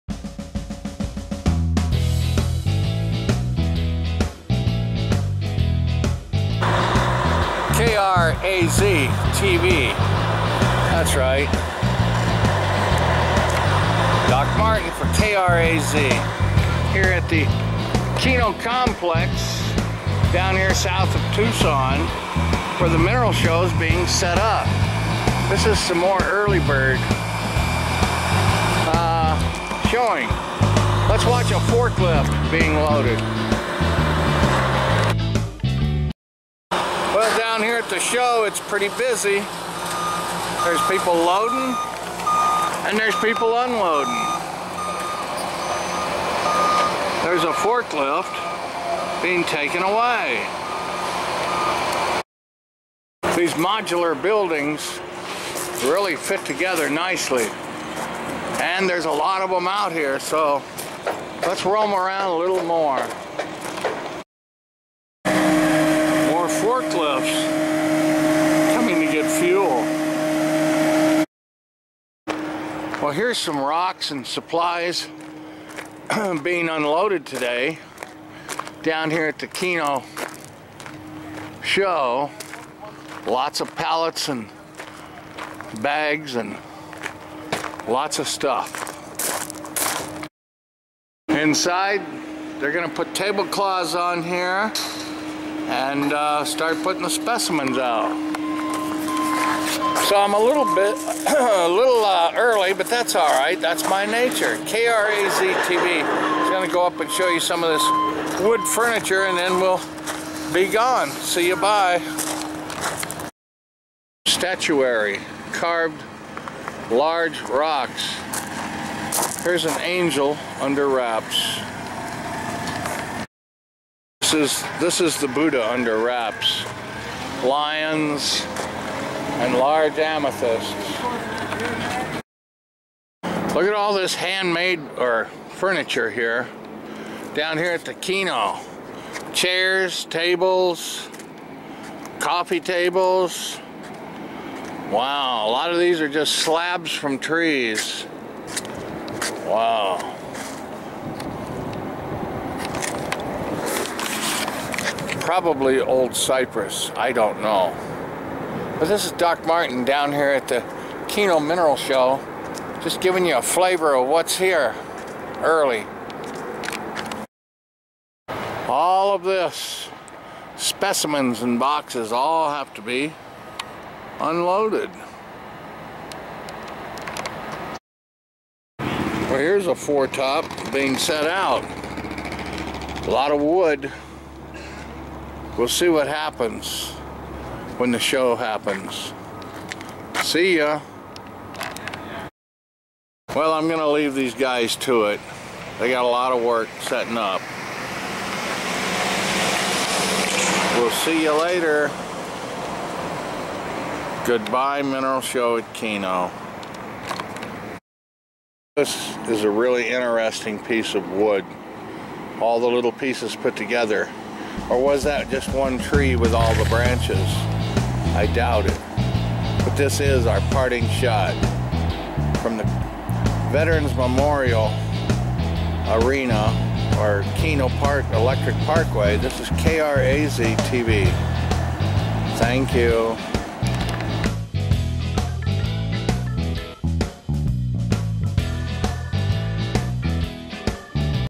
Kino Mineral Show Setup
KRAZTV shows Kino Electric Park setup for Mineral Shows in Tucson,Arizona.